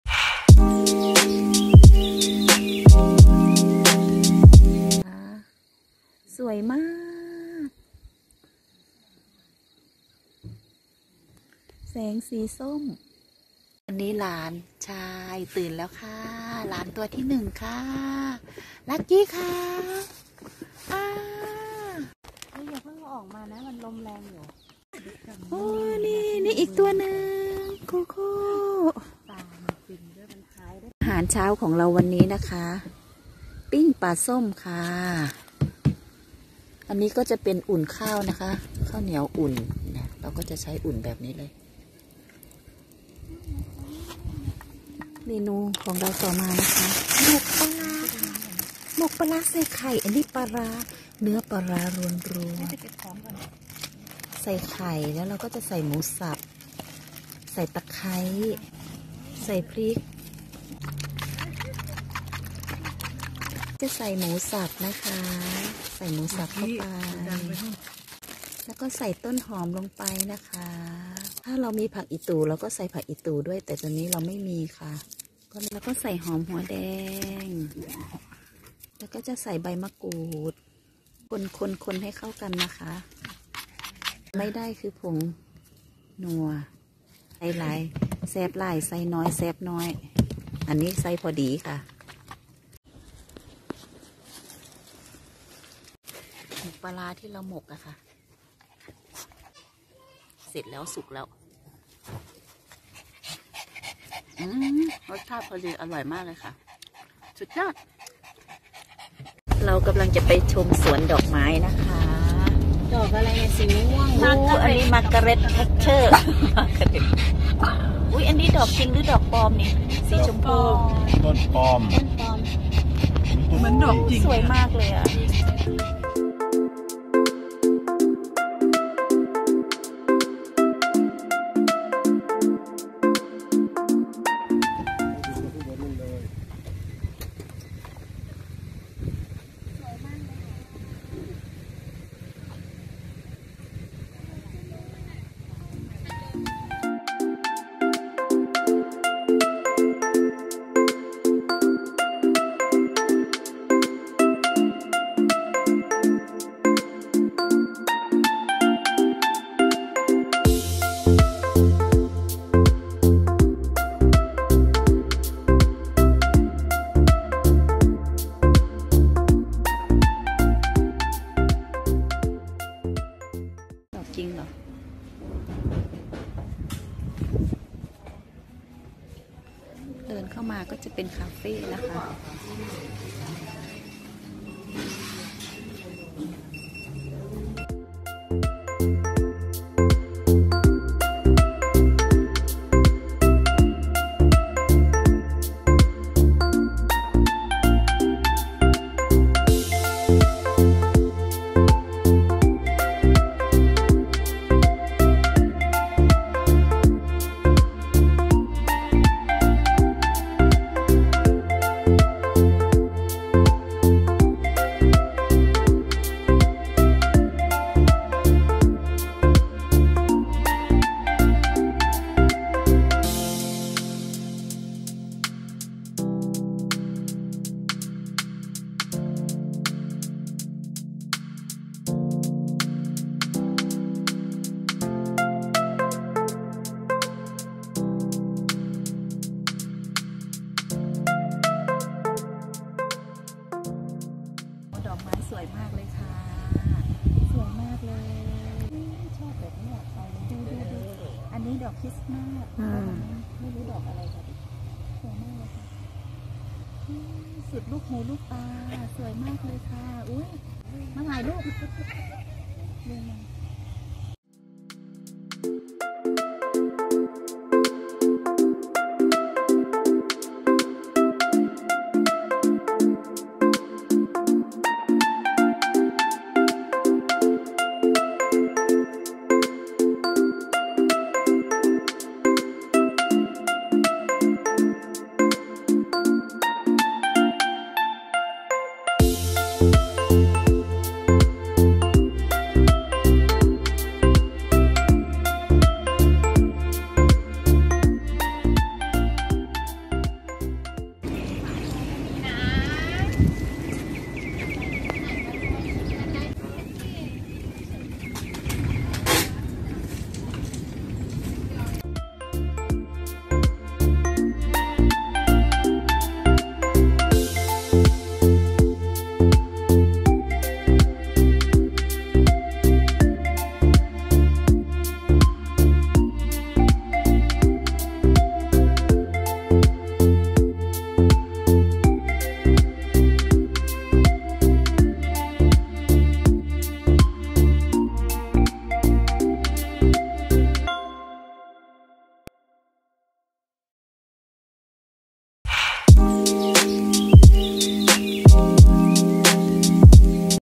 เที่ยวเขาตะเคียนโง๊ะ-ภูทับเบิก กับคุณนายบ้านนา EP.2
วันที่สองของการเดินทาง คุณนายชอบมากๆเลย แฮปปี้สุดๆ